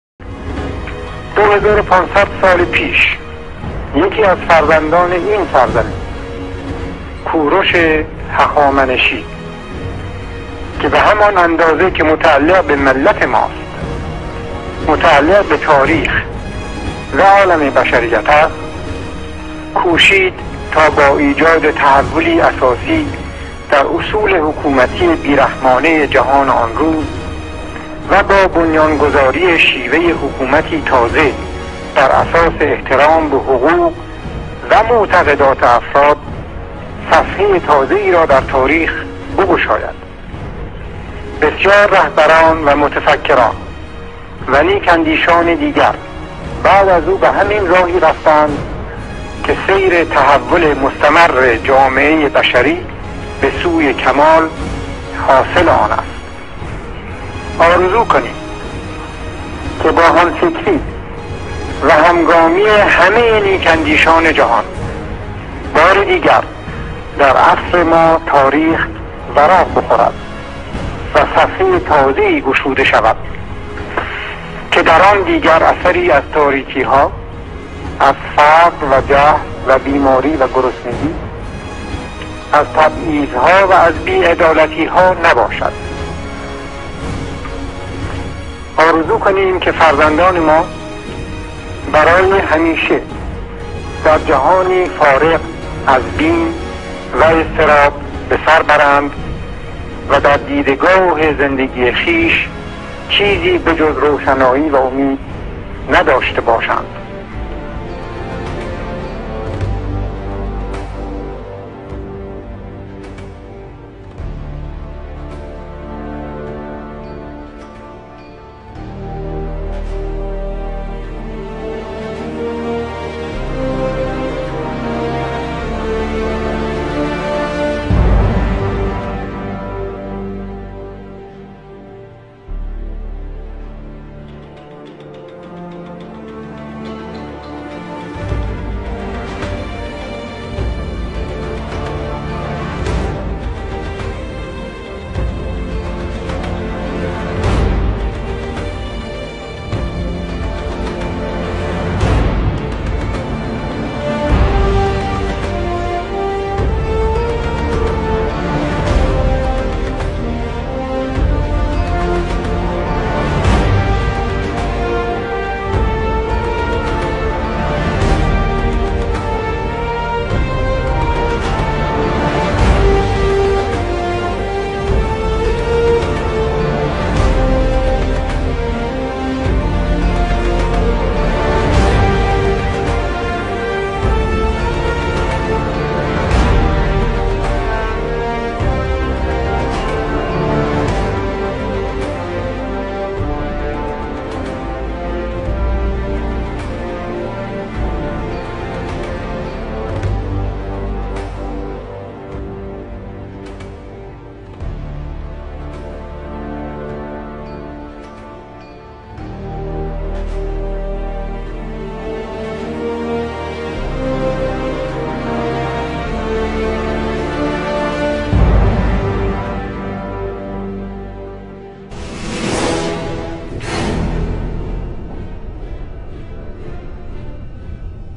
‫ تمدن بزرگ ‬
سخنان پهلوی دوم در مورد کورش بزرگ به همراه فیلمی کمیاب ازسفر به ایالات متحده آمریکا